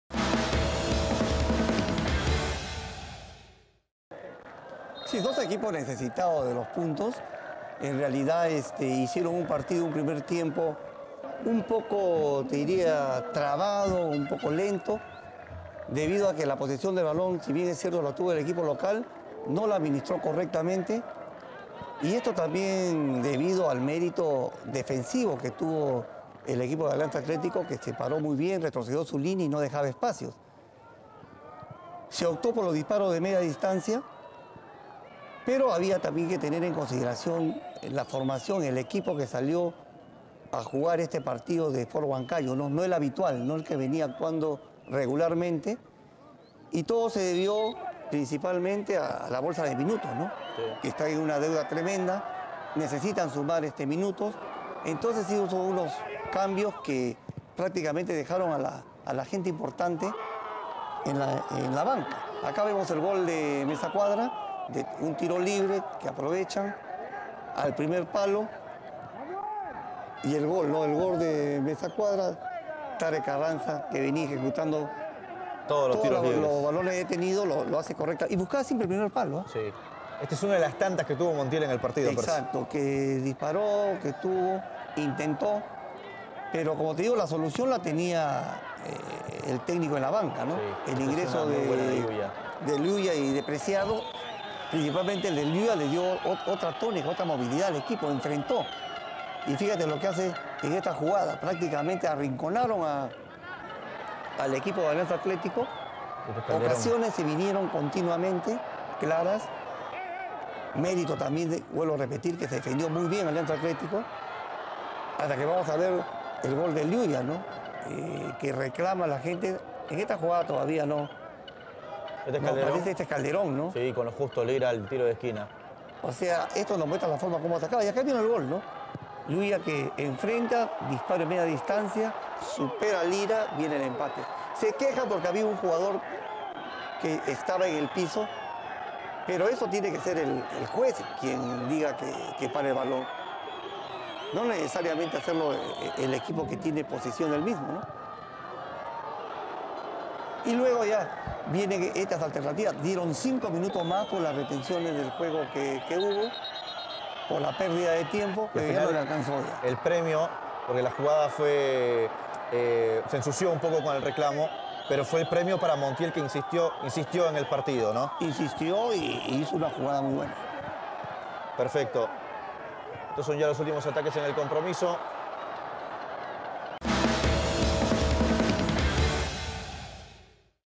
Resumen: Sport Huancayo vs. Alianza Atlético (1-1)
El 'Rojo Matador' igualó 1-1 con el conjunto sullanense y sumó su tercer empate consecutivo, cayendo al sexto lugar del Torneo Clausura.